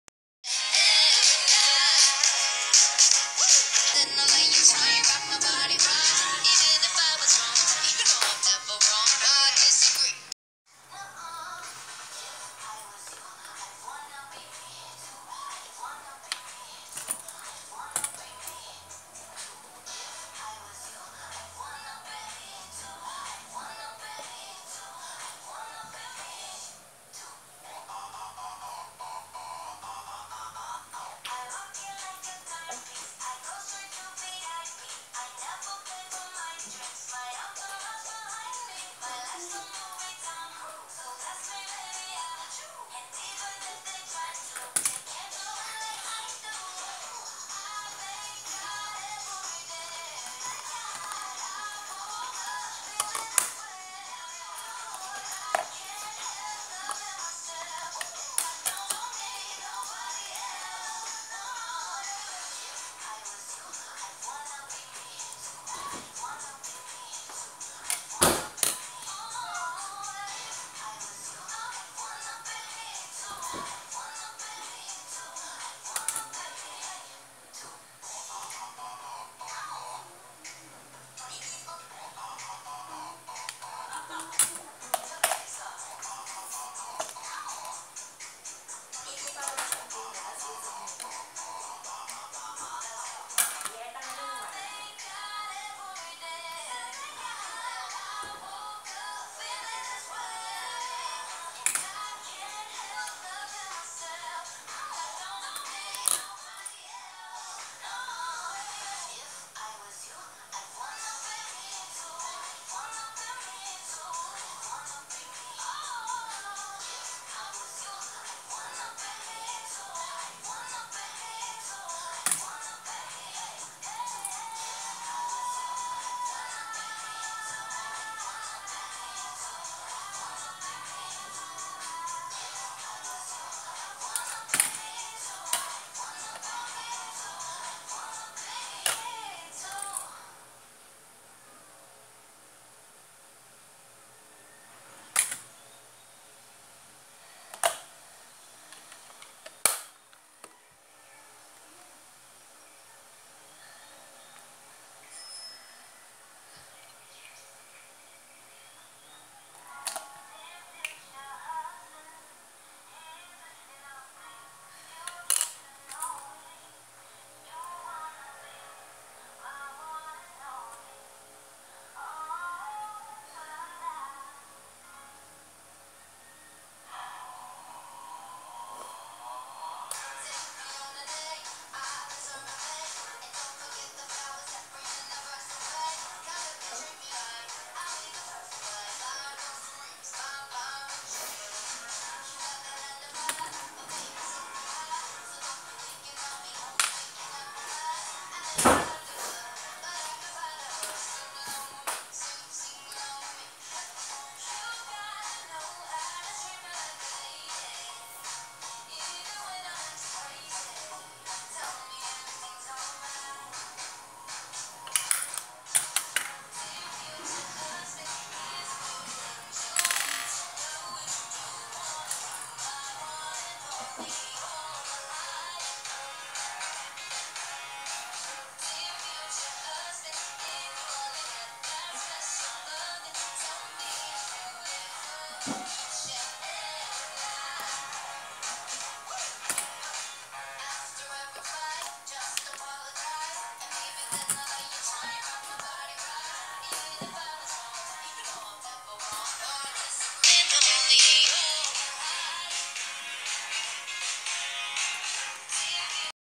Makeup routine ft. Princesbunyface
In this vid I (princesbunyface) will show you my makeup routine

Editor:Catwarrior
Producer:Princesbunyface
Made by: 4 musketeers